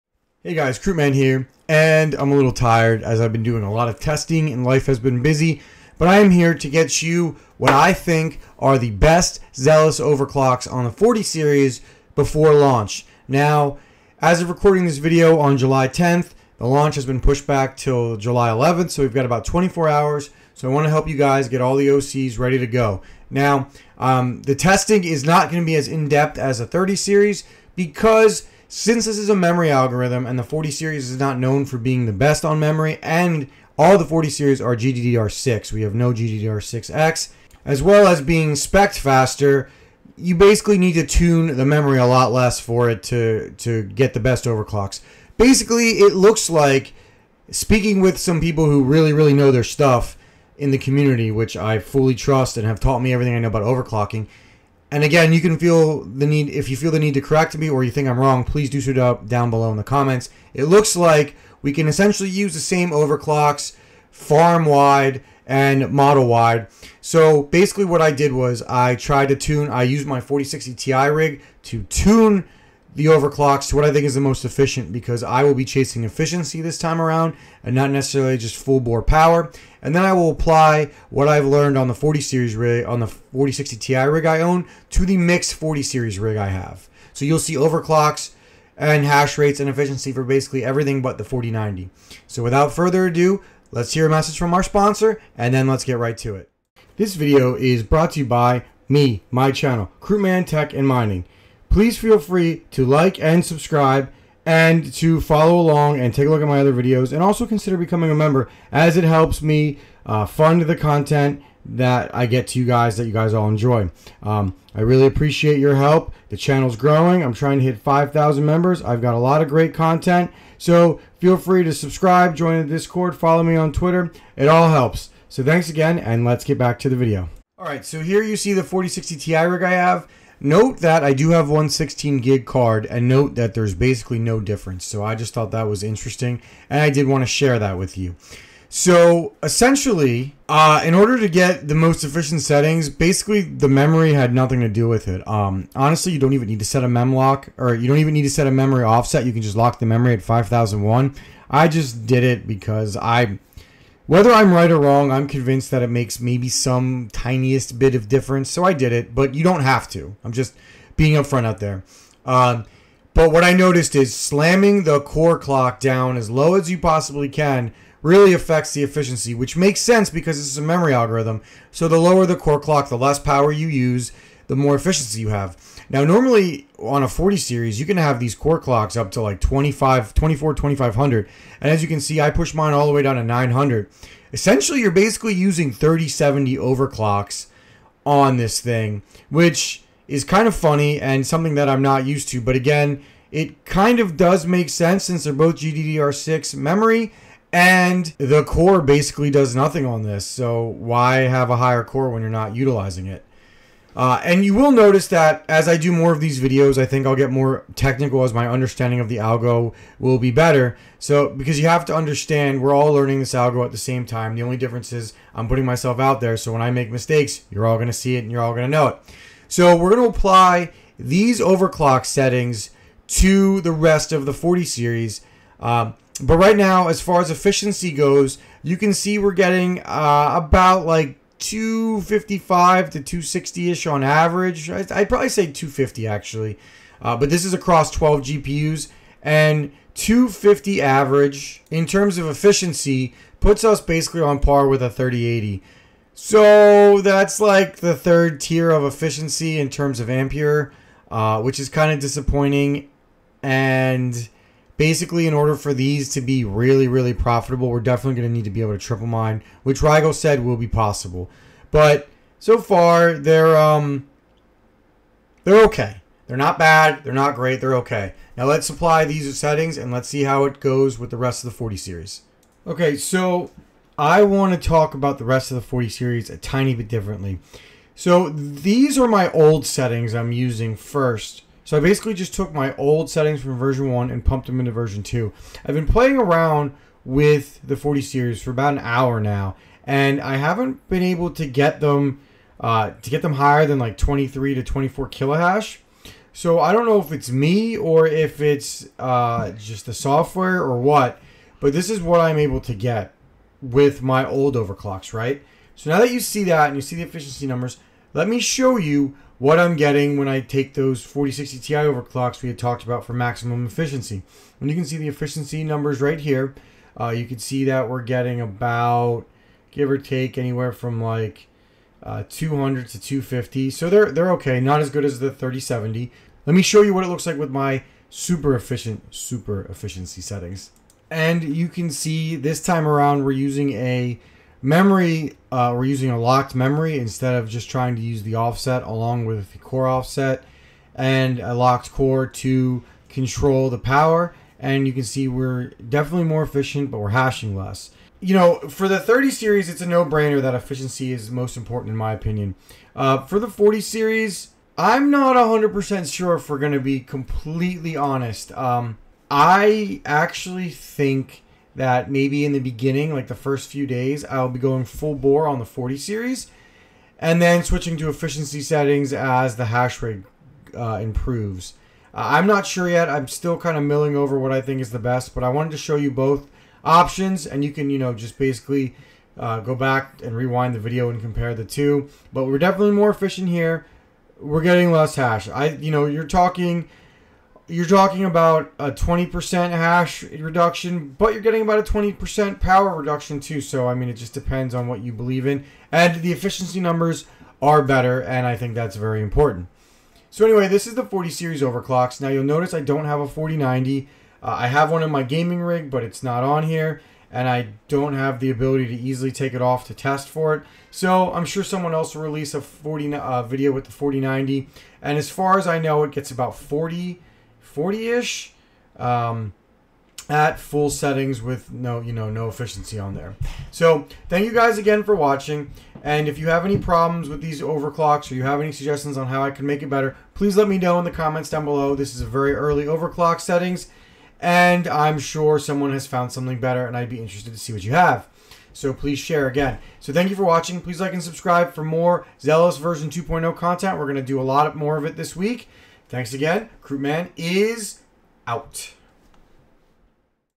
XELIS V2 (2.0) 40 SERIES OVERCLOCKS and HAHSRATE TESTING!
In this video ill share my 40 series overclocks (no 4090 sorry guys) and explain how I got them and what settings ill be using at launch!

Thank you to VPIOR.NET for providing the test net, go check them out!

My preferred pool is k1 pool